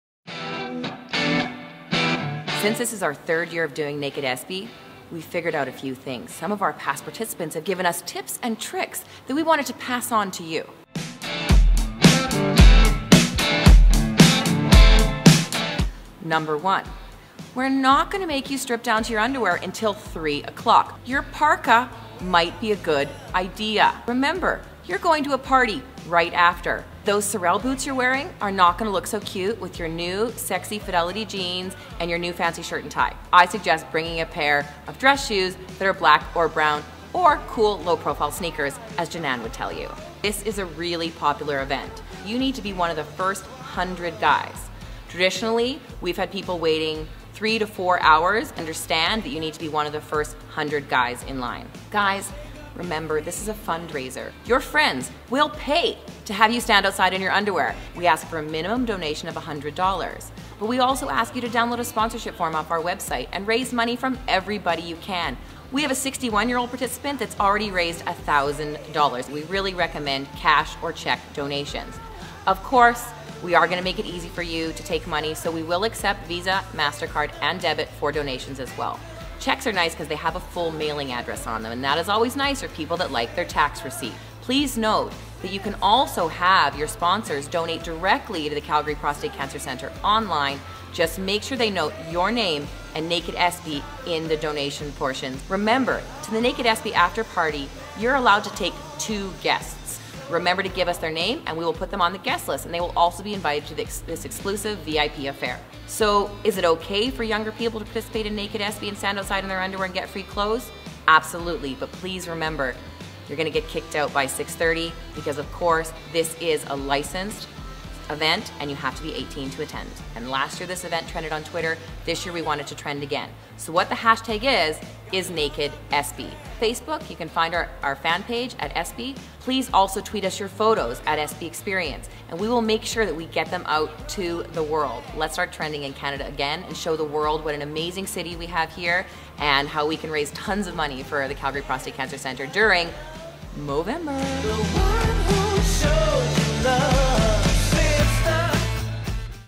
nakedespy 2012 Need To Knows
nakedespy 2012 tips and tricks from espy stylists and past participants. 3pm November 17, 2012